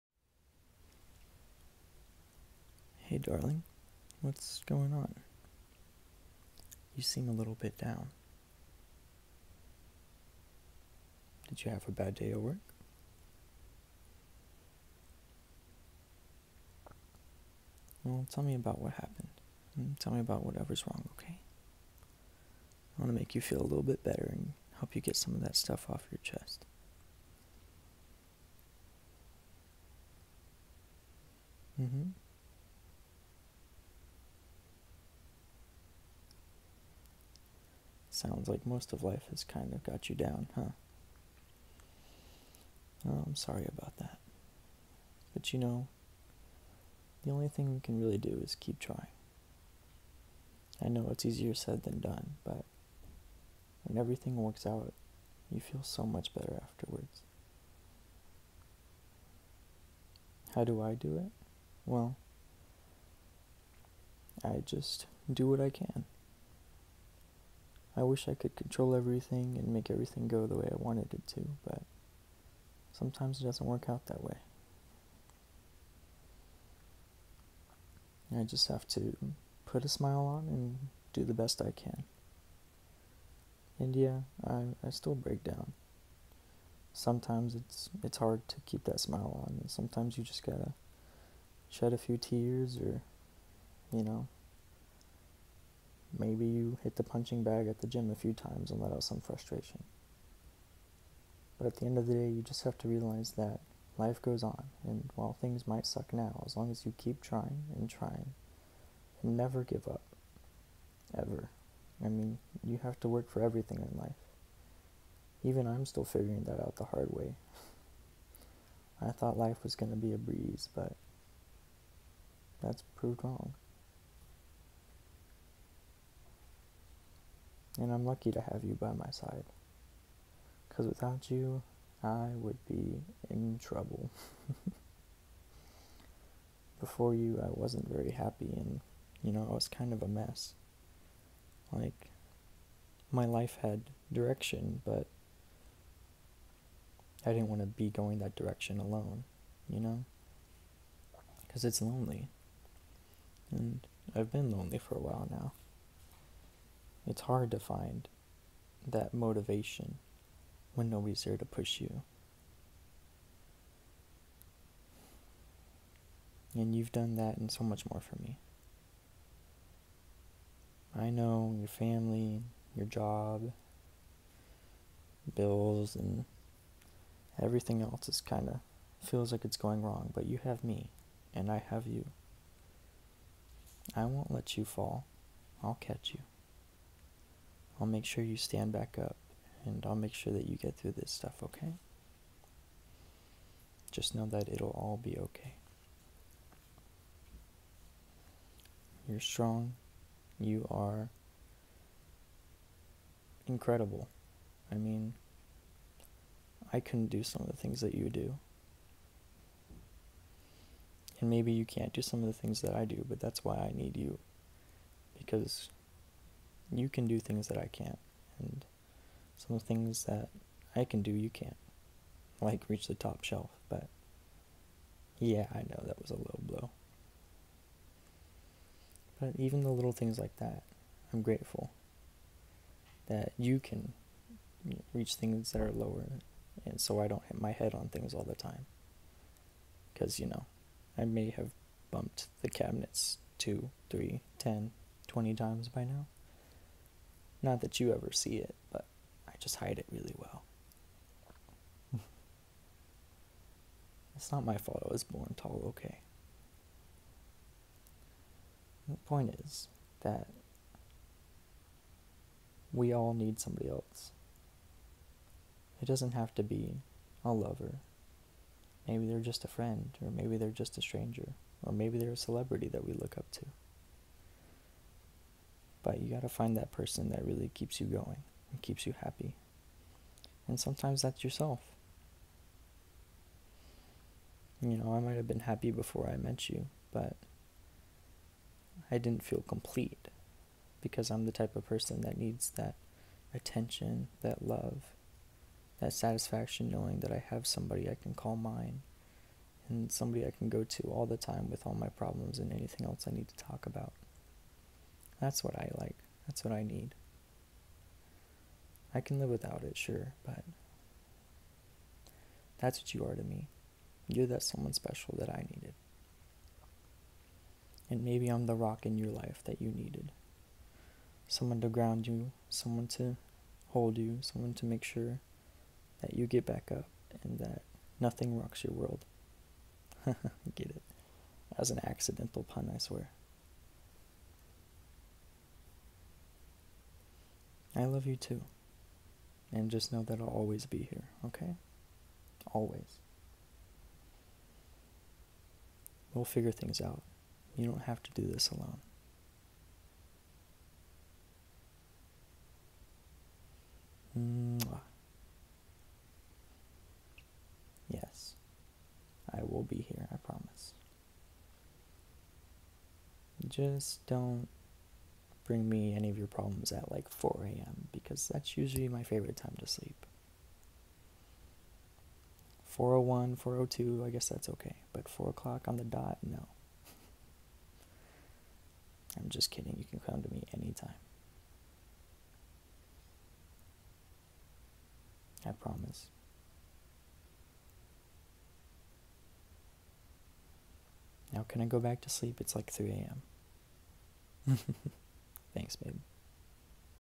[ASMR Roleplay] Boyfriend Supports You When Life Gets You Down
Super sorry that this is a late upload! Life kinda had me down for the past several days so I wasn't feeling motivated, but I had some good news today and thought that maybe you guys might be having a hard time too and needed some support. Hopefully this made you feel better or made ya giggle once or twice~

P.S. You guys blew up my last video so thank you everyone, it honestly surprised the heck out of me and blew my mind